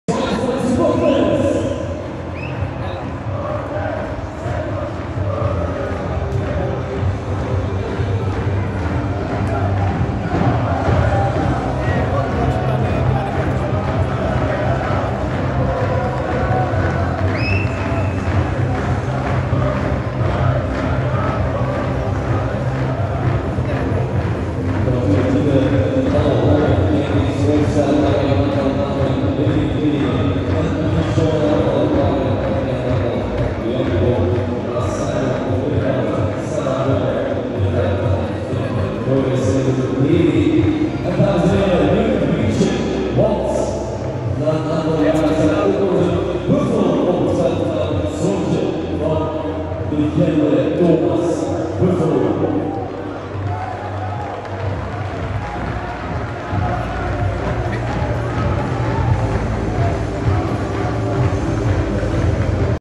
Pre-season 2022: Ο Μάνταλος Πήγε στους Φιλάθλους μετά το Γκένκ - ΑΕΚ | enwsi.gr
Όλα τα νέα της ΑΕΚ από το Άλκμααρ της Ολλανδίας, όπου γίνεται το βασικό στάδιο της προετοιμασίας της για την σεζόν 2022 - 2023. Σε αυτό το διάστημα η ΑΕΚ θα δώσει τρία φιλικά παιχνίδια με την Γκενκ στις 16/07, με την Φόλενταμ στις 22/07 και με την Ουτρέχτη στις 27/07.

Μείνετε συντονισμένοι για να μαθαίνετε πρώτοι τις τελευταίες εξελίξεις της ομάδας.